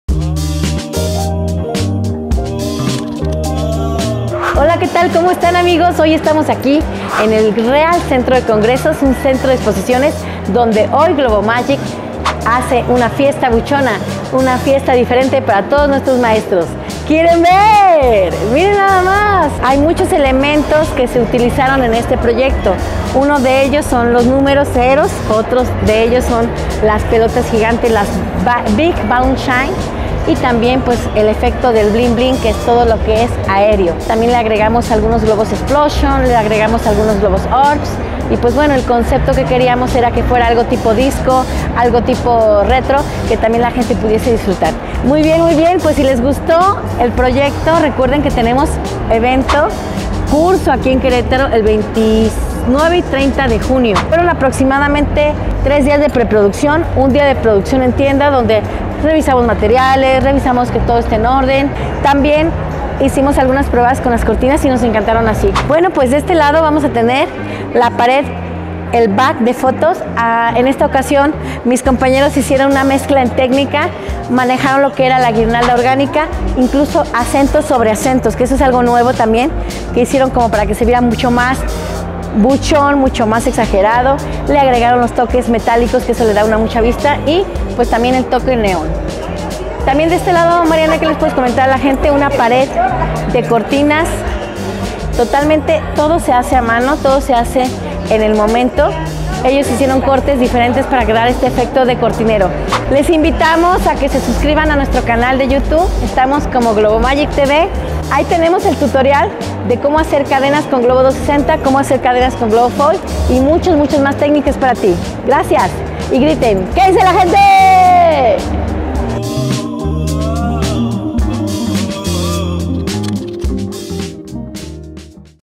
¡Mira este montaje que hicimos para el día del maestro! 👨🏻‍🏫👩🏻‍🏫🎈
¿Que tal amigos? 🎈
Acompañanos a ver el proceso de este maravilloso montaje que hicimos para el día del maestro. 👨🏻‍🏫👩🏻‍🏫
Y de igual manera para invitarlos a que se inscriban a nuestro próximo curso que será este 29 y 30 de Junio en donde aprenderás muchas cosas, entre ellas técnicas que te serán muy útiles para tus decoraciones. 🥳